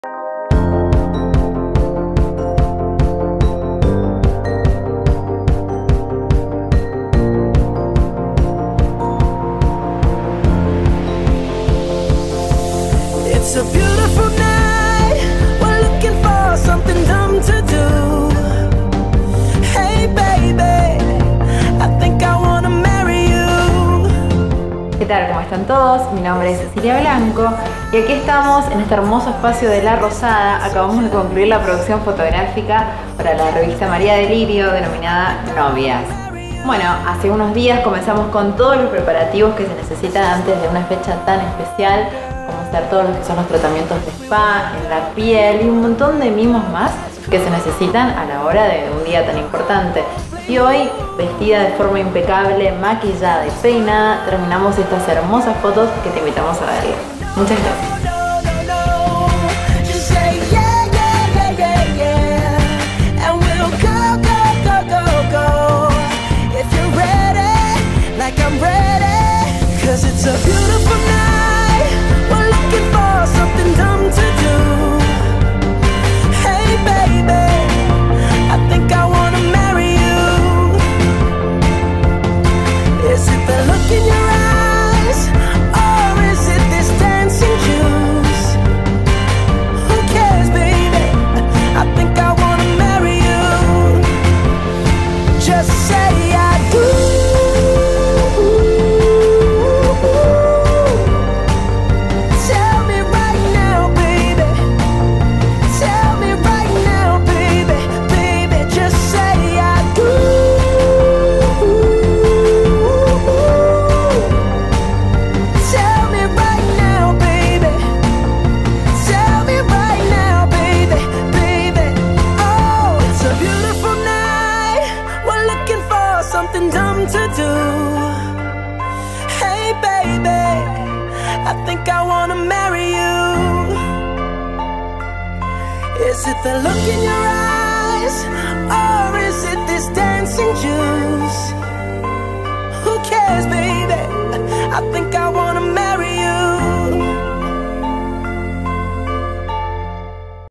MARIA DELIRIO NOVIAS
PRODUCCIÓN MARIA DELIRIO
‪#‎BRIDEMDTV‬, ‪#‎MDTV‬, ‪#‎MODABRIDE‬
Modelo: Ceci Blanco
Estilismo: María Aidée - Centro Integral de Belleza
Vestido: Lara Diseños Alta Costura
Accesorios: Magu Accesorios
Rosario en mano: RO Servin
Zapato: Borse Ctes
Locación: La Rozada Suites
Producción: Las Gusbeth